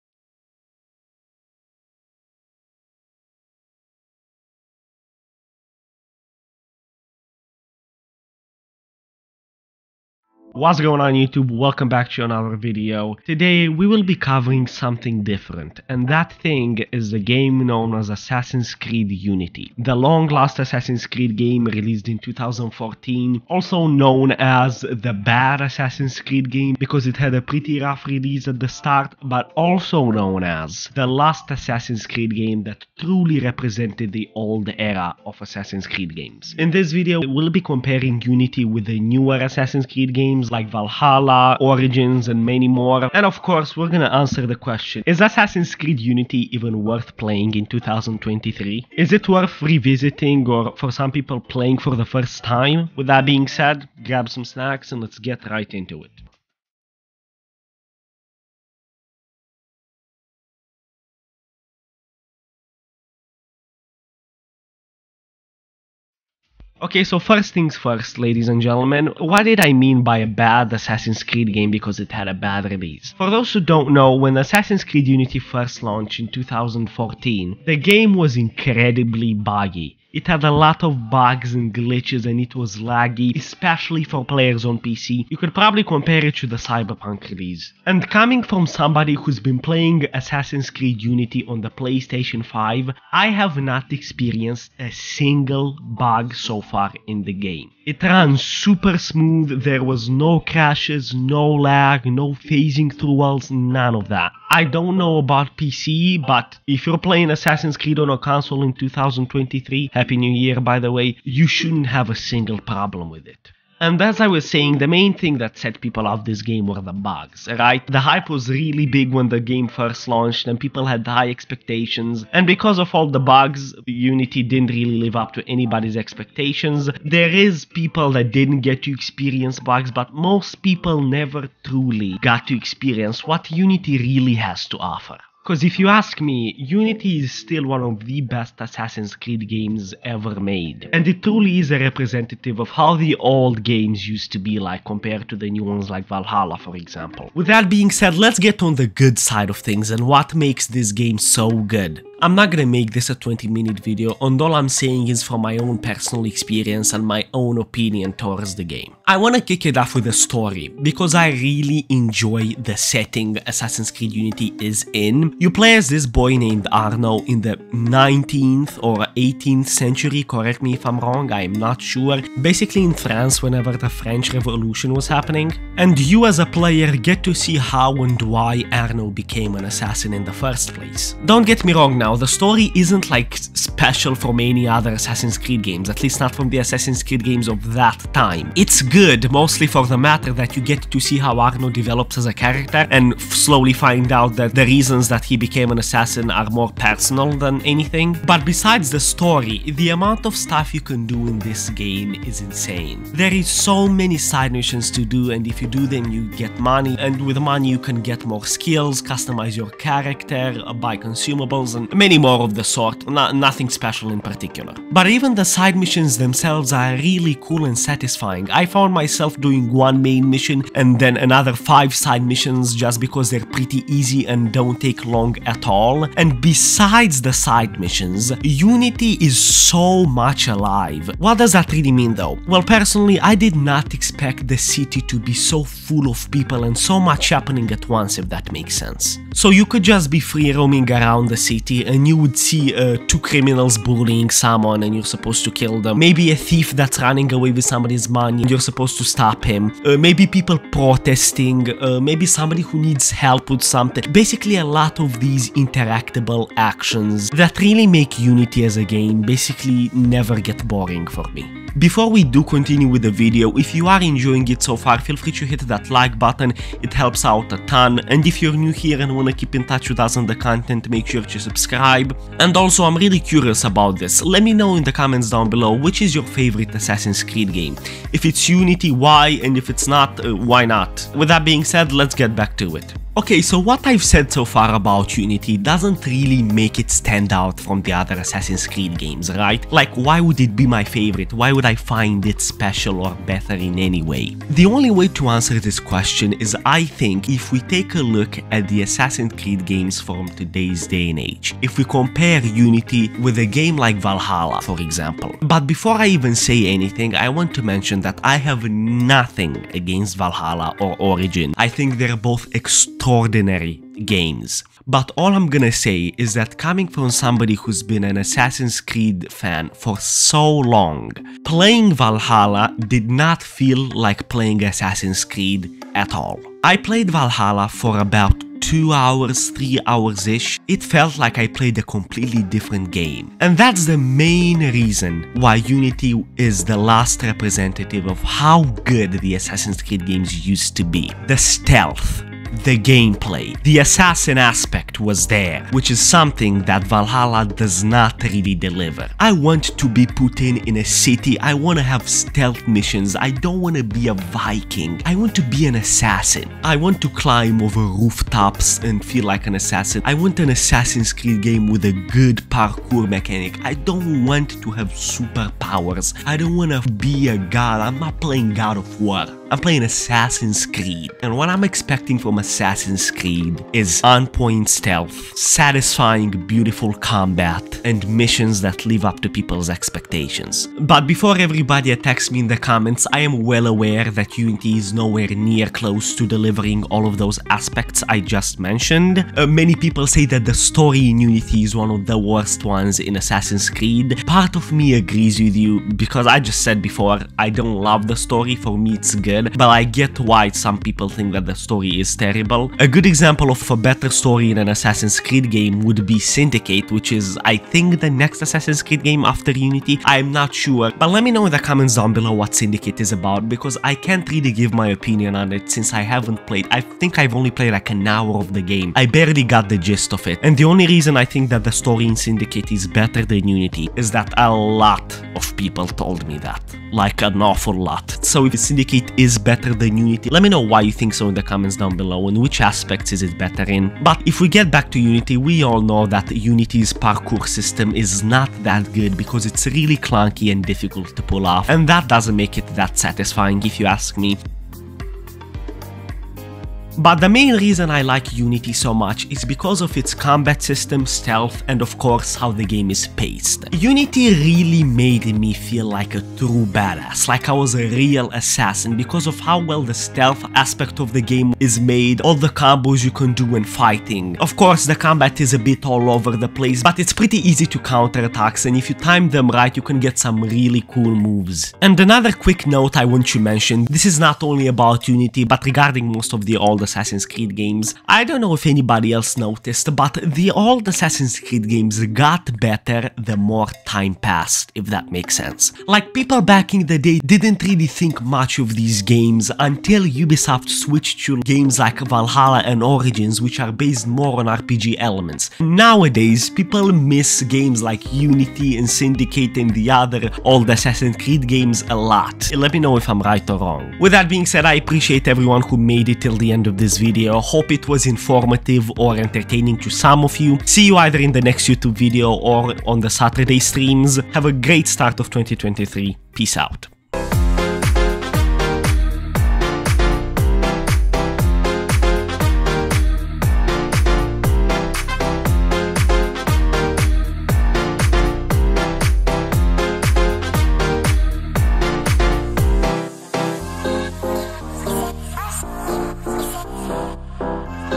Is Assassin's Creed Unity worth playing in 2023?
Assassin's Creed Unity in 2023 - In this video we are covering Assassin's Creed Unity and answering the question: Should you play it in 2023? The 2014 release by Ubisoft had a terrible start because of all the bugs but nowadays it's considered to be one of the best AC games ever made and for good reason too. Unity is the last game that truly represents how the old Assassin's Creed franchise used to be before Ubisoft added more RPG elements it, which we can see in games like Valhalla and Origins. The combat system, the stealth and the pacing of the game are just a few elements that make Unity as good as it is today. Make sure to watch till the end if you're interested in finding out our full opinion on the game and if you want to know if Unity is worth revisiting in 2023.